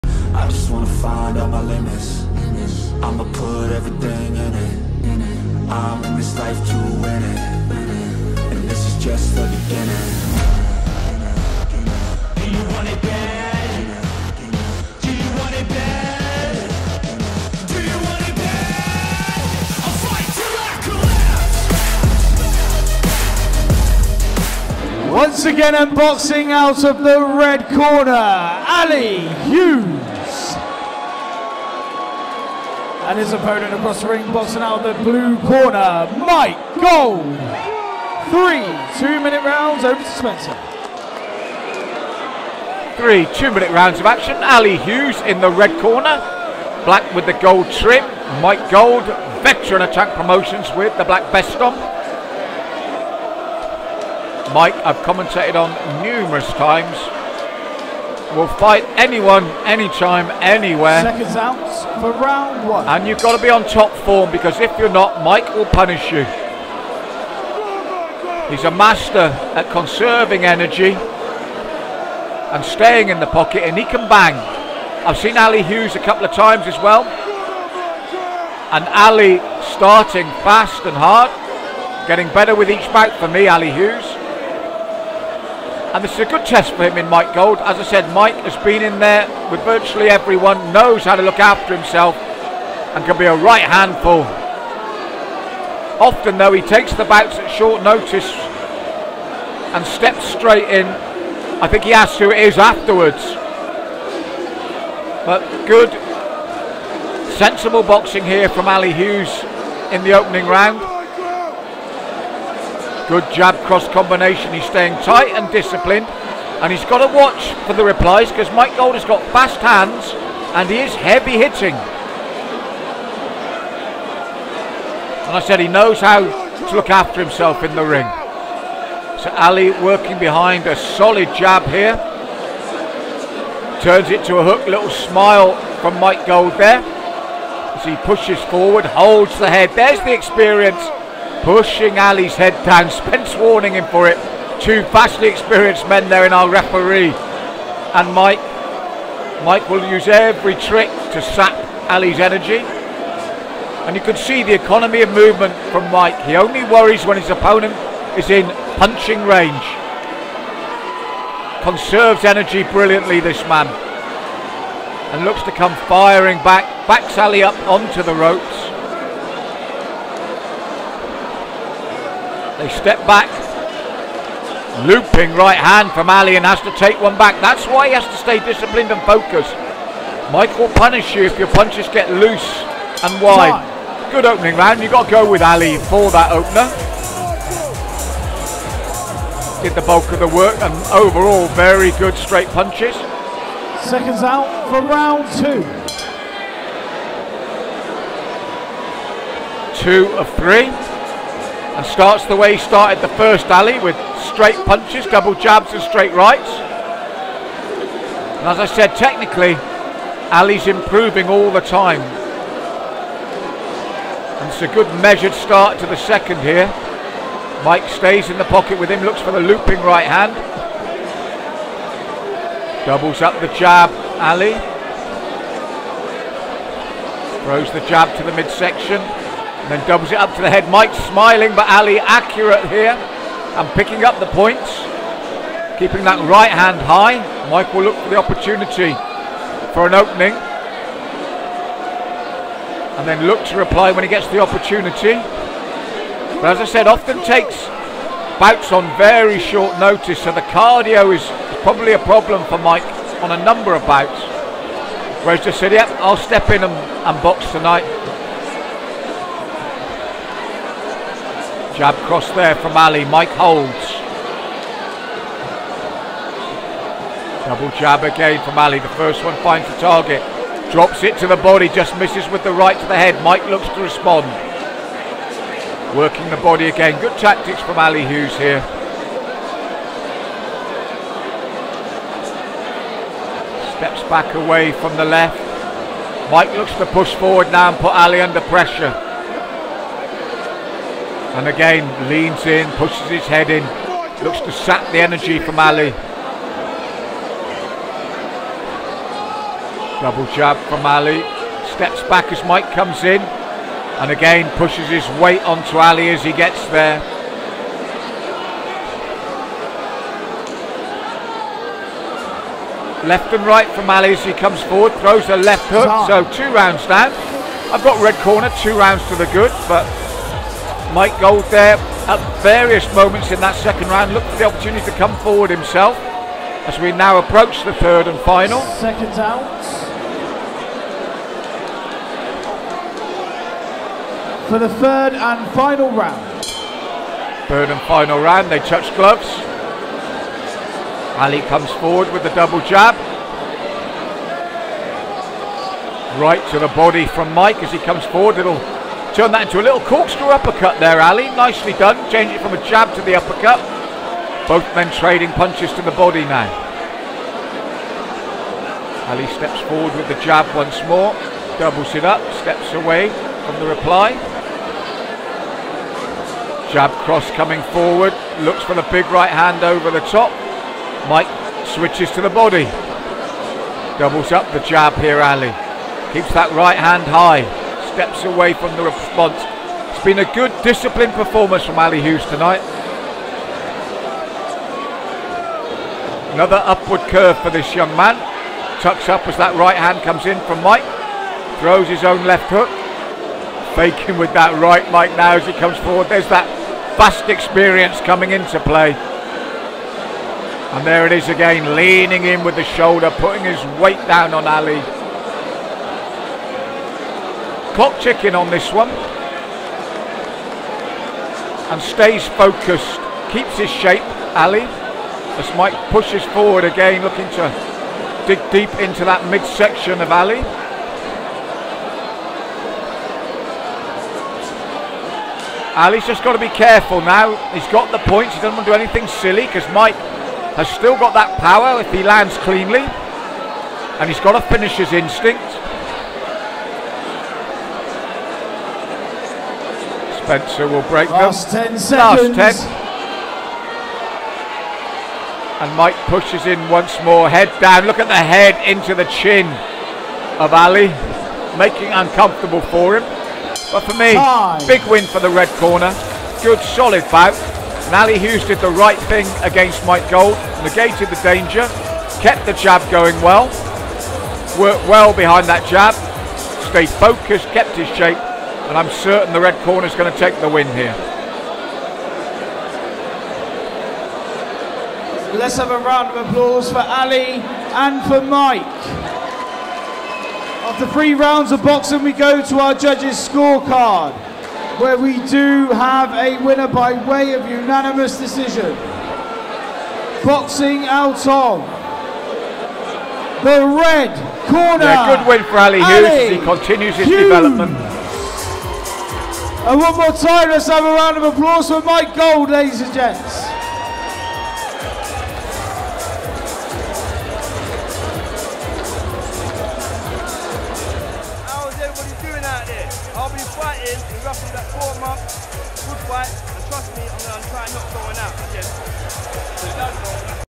Tank Promotions Bout 14 Ali Hughes Vs Mike Gold
Tank Promotions
Heavyweight Prizefighter
Bout 14
Ali Hughes Vs Mike Gold
Swindon, Meca
25th February 2023
Commentary: Malcolm Martin